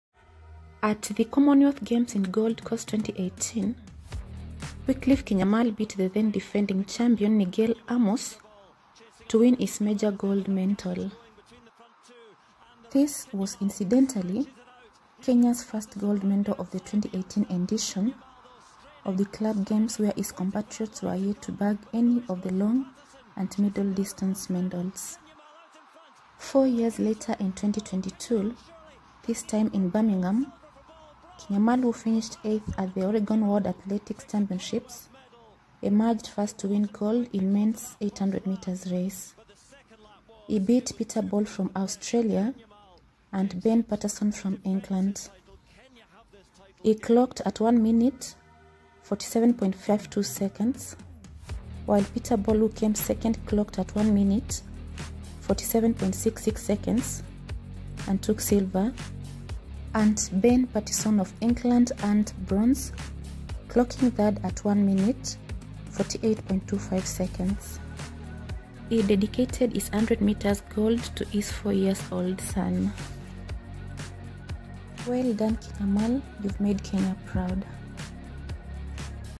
KINYAMAL 🇰🇪 wins Gold in Men's 800m Final|| Commonwealth Games 2022
At the commonwealth games in gold coast 2018, Wycliffe Kinyamal best the then defending champion Nijel Ams to win his major gold medal.

This was incidentally kenya's first gold medal at the club games where his compatriots were yet to bag any of he long and middle distance medals.

Four years later in 2022, this time in Birmingham Kinyamal who finished 8th at the Oregon World Athletics championships, emerged first to win gold in men's 800m race.
He beat Peter Bol from Australia and Ben Patterson from England.

He clocked at 1:47.52 seconds, while Peter Bol who came second clocked at 1:47.66 seconds and Ben Pattison of England earned Bronze clocking third at 1:48.25 seconds.

He dedicated his 800m gold to his 4years old son.